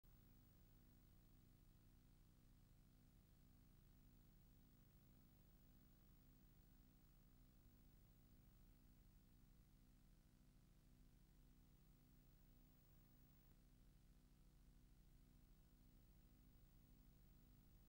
Obe Postma op Gysbert Japicxpriis-útrikking 1950
De dichter Obe Postma komt út it stêdhûs fan Boalsert nei de útrikking fan de Gysbert Japicxpriis en de Joast Halbertsmapriis yn 1950.
Postma hie sels de G.J.-priis yn 1947 krigen.